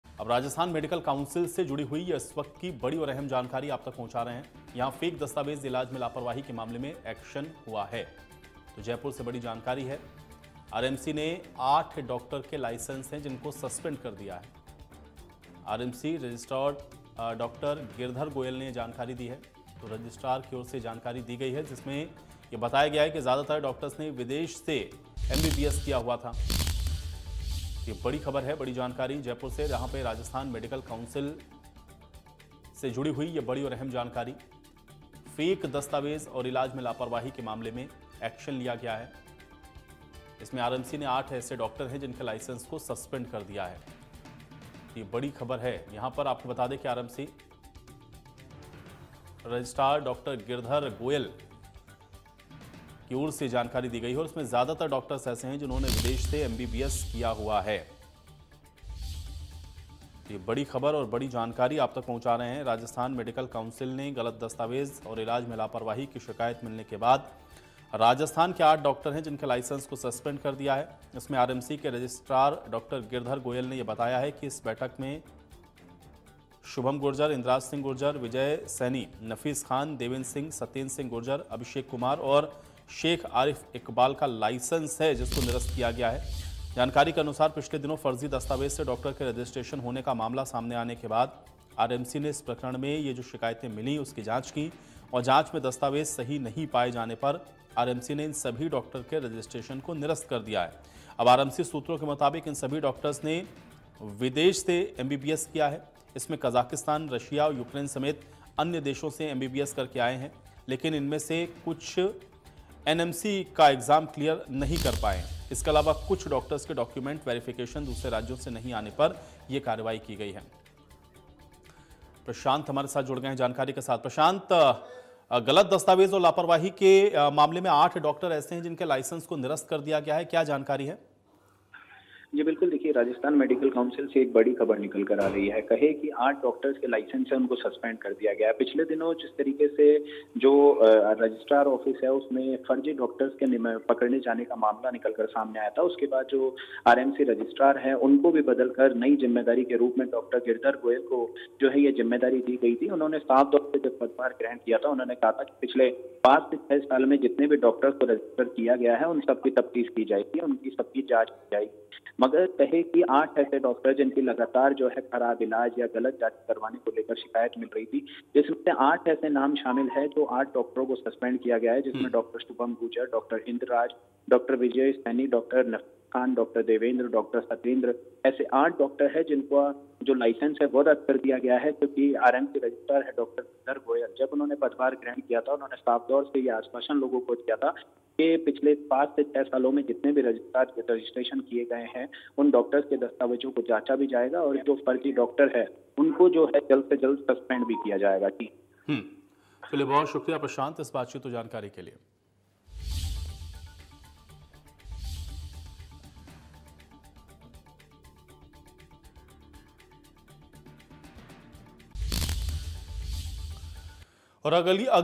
Jaipur News: इलाज में लापरवाही मामले पर एक्शन, RMC ने 8 डॉक्टरों के लाइसेंस किए सस्पेंड | India News
राजस्थान मेडिकल काउंसिल से बड़ी खबर, फेक दस्तावेज, इलाज में लापरवाही मामले पर एक्शन, आरएमसी ने 8 डॉक्टरों के लाइसेंस किए सस्पेंड, RMC रजिस्ट्रार डॉ गिरधर गोयल ने दी जानकारी, ज्यादातर डॉक्टर्स ने विदेश से की थी एमबीबीएस.